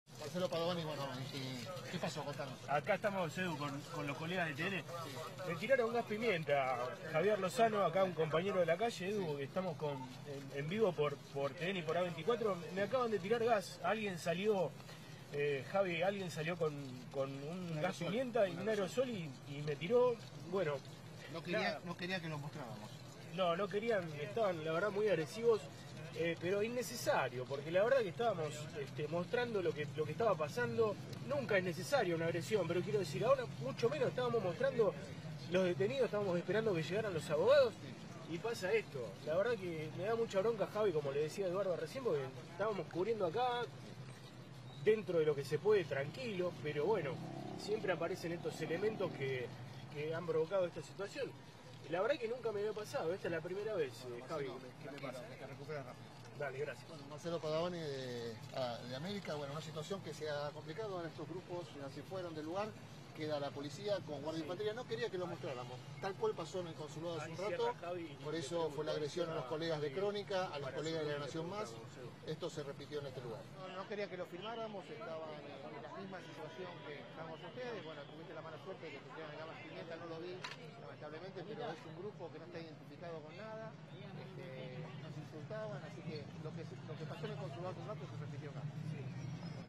Protesta violenta en el consulado de Chile: le tiraron gas pimienta a un periodista de A24
Marcelo Padovani, el periodista de A24 atacado en medio de las protestas frente al consulado de Chile en Buenos Aires, donde hubo violencia y agresiones a periodistas. A Marcelo Padovani le tiraron gas pimienta en los ojos: "Alguien salió con un aerosol de gas pimienta y me tiró. Estaban mu agresivos. Estábamos mostrando a los detenidos, esperando a los abogados. Estábamos cubriendo". También atacaron a periodistas de  LN+